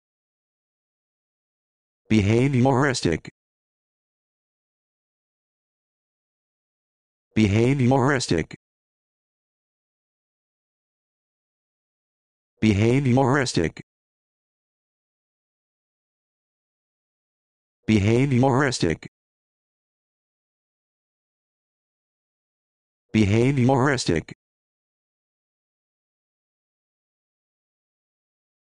How to Pronounce Behavioristic
This video shows you how to pronounce Behavioristic